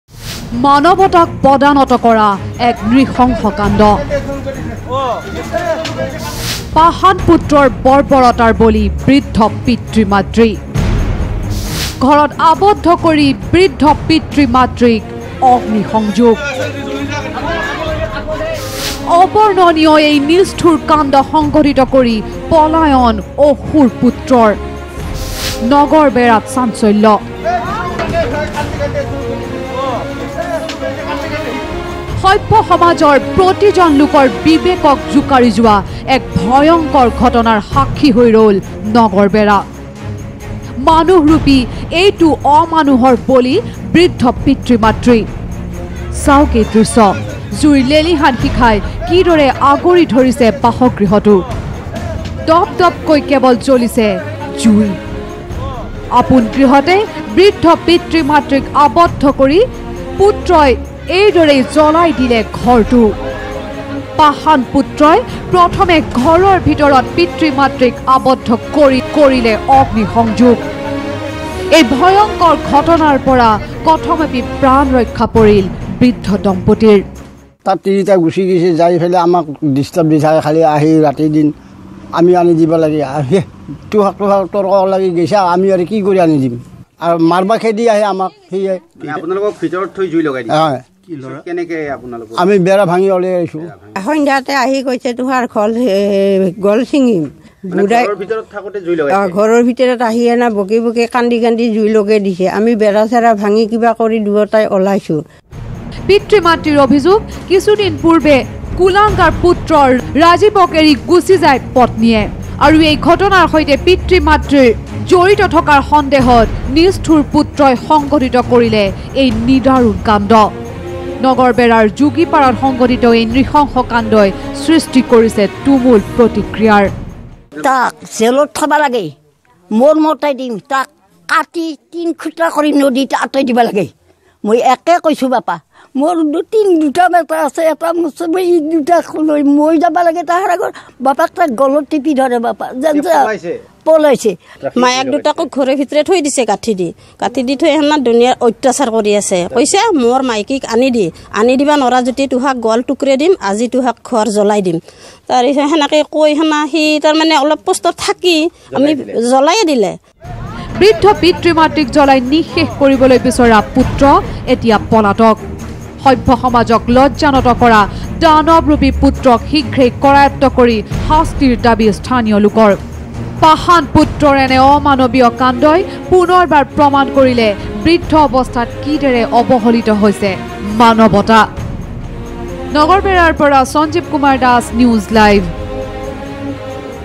পিতৃ-মাতৃক আৱদ্ধ কৰি ঘৰত অগ্নিসংযোগ পাষাণ পুত্ৰৰ।
পুনৰ ভূলুণ্ঠিত হ’ল মানৱতা। নগৰবেৰাত পাষাণ পুত্ৰৰ বৰ্বৰতাৰ বলি হ’ল বৃদ্ধ পিতৃ-মাতৃ। পিতৃ-মাতৃক আৱদ্ধ কৰি ঘৰত অগ্নিসংযোগ পাষাণ পুত্ৰৰ। নিষ্ঠুৰ কাণ্ড সংঘটিত কৰি পলায়ন পুত্ৰৰ। কথমপিহে প্ৰাণ ৰক্ষা পিতৃ-মাতৃৰ।